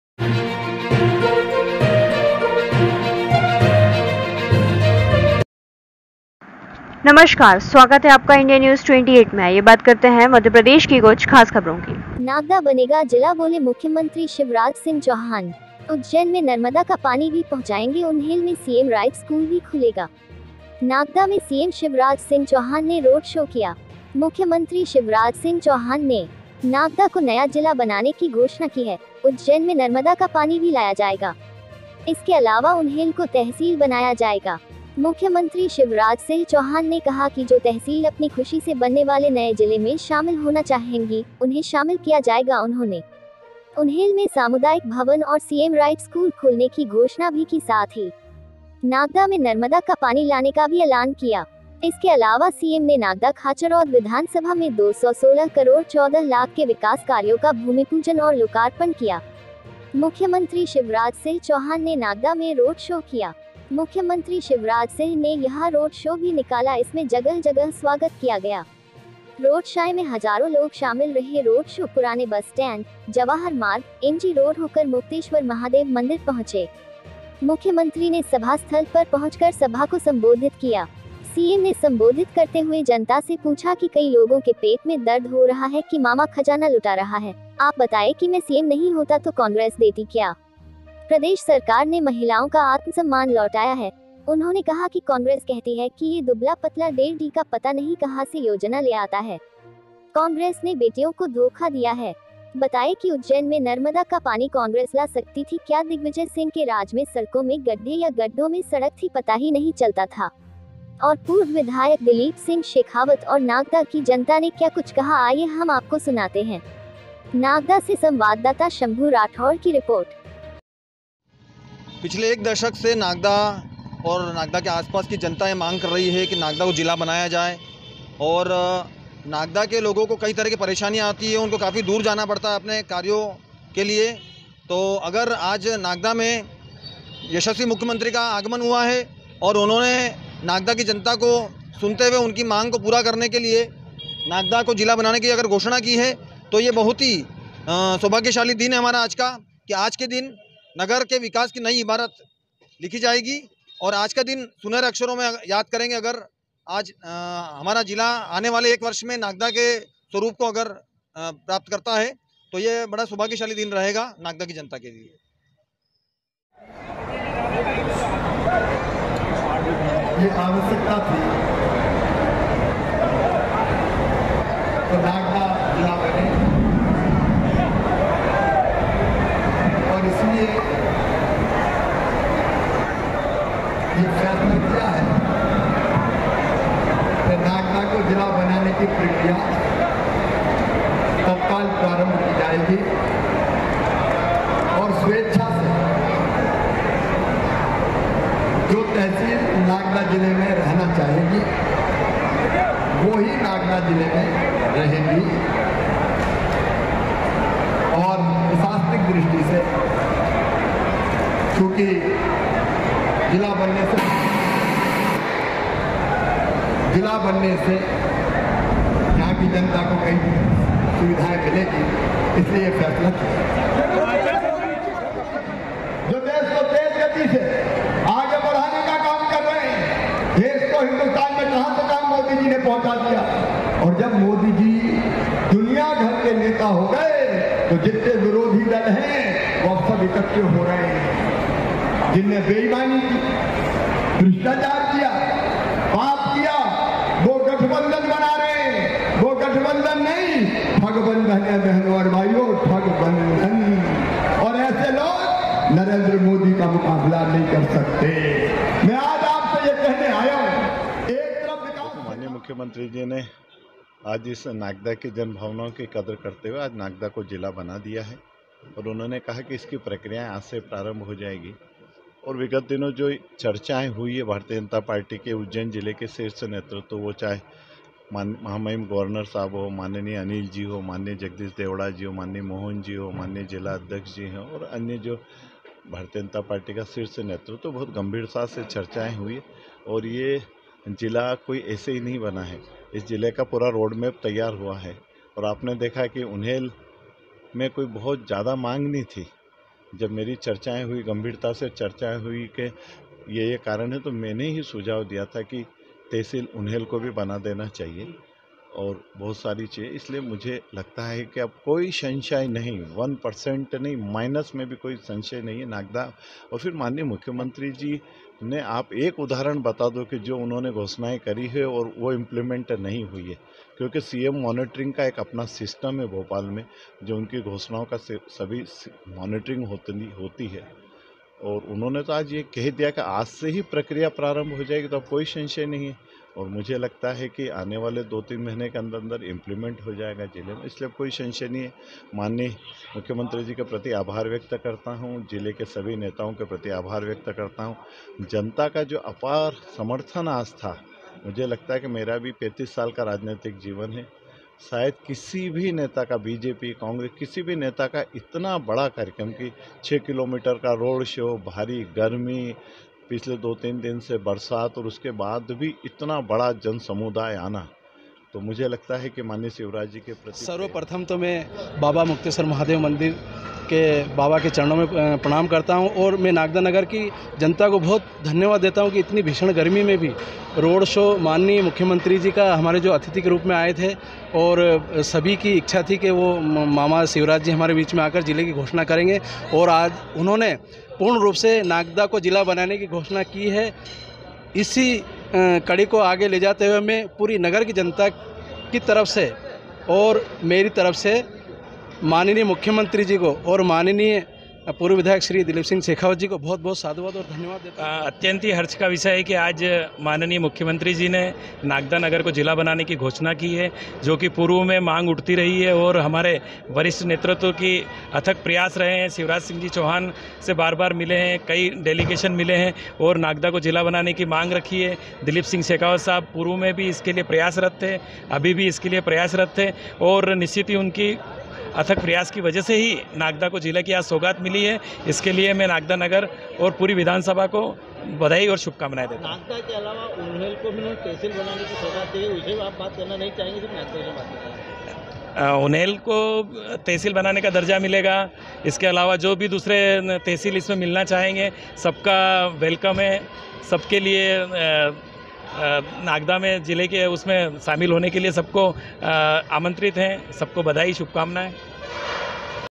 नागदा बनेगा जिला बोले मुख्यमंत्री शिवराज सिंह चौहान
Official